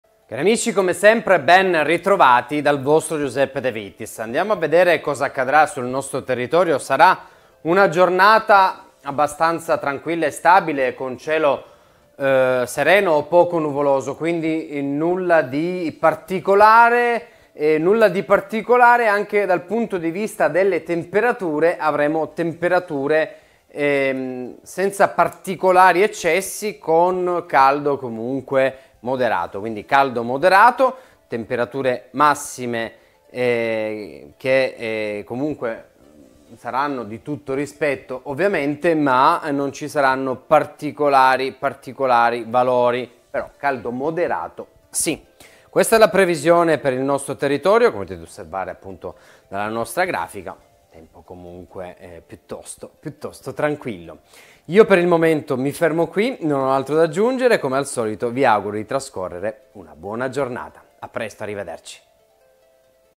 Anticiclone africano protagonista sull'Italia
Quadro meteo stabile con bel tempo, tanto sole e poche nubi su tutte le regioni. Molto caldo con afa diffusa